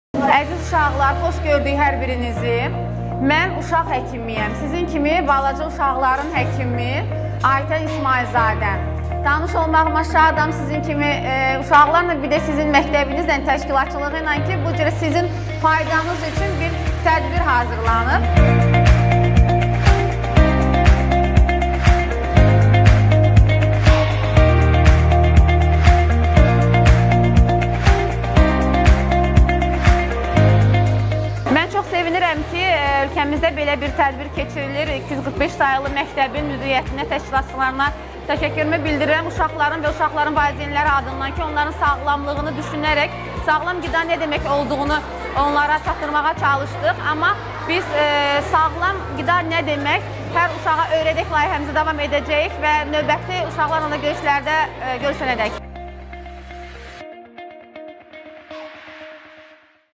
245 sayli mektebde saglam qidalanma ile bagli seminar - Hekim - pediatr Ayten İsmayilzade
Pediatr Aytən İsmayılzadə - 245 saylı məktəbdə sağlam qidalanma.